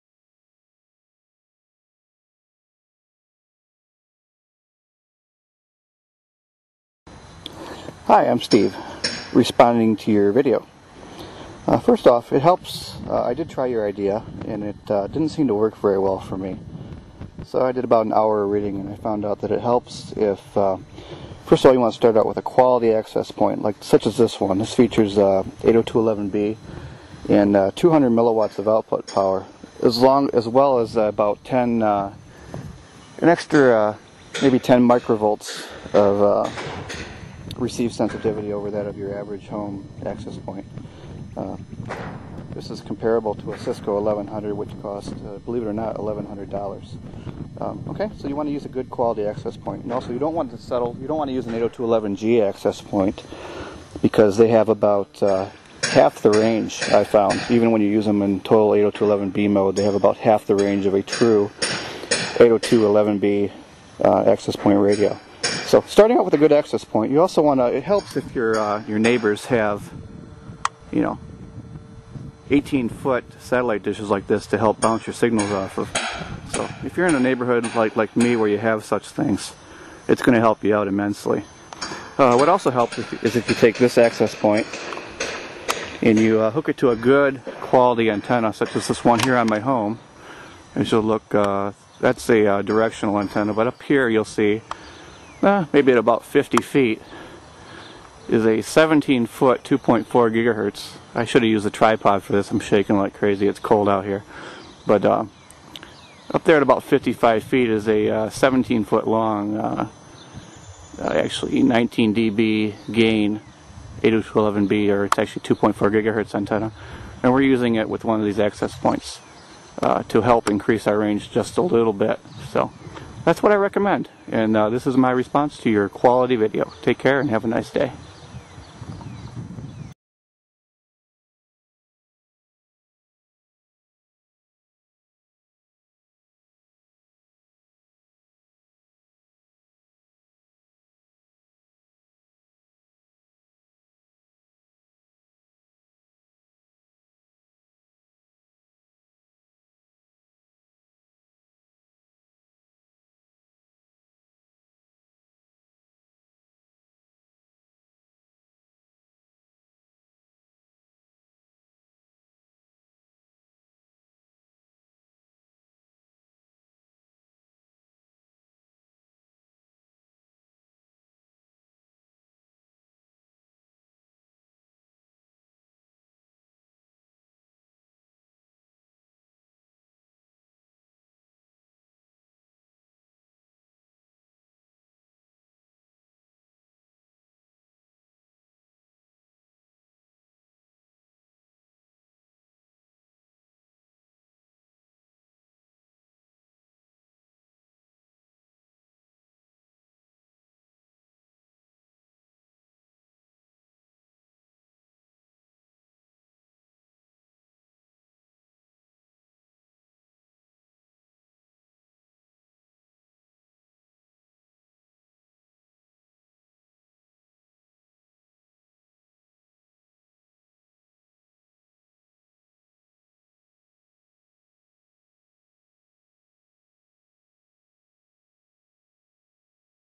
Re: How to Increase Your Wi-Fi Signal
Just a Quick response!
Note: the arrangement is also used as a
wireless client device! not only an
access point.
Everyone should own one of these.